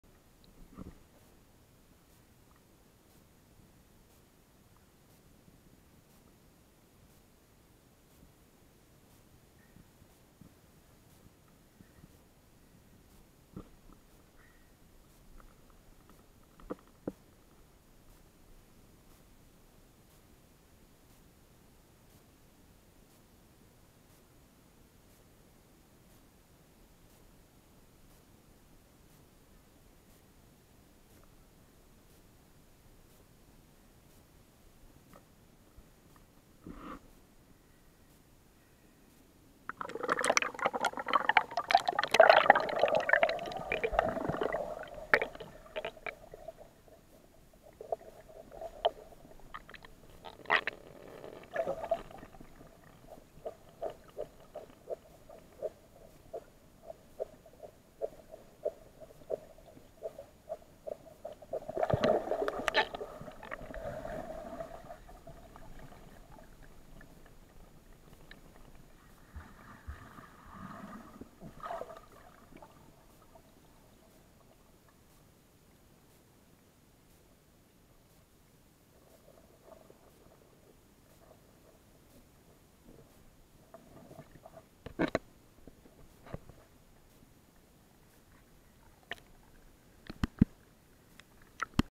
Isola Bisentina... che acque!
Le splendide acque intorno l'isola Bisentina nel lago di Bolsena (VT)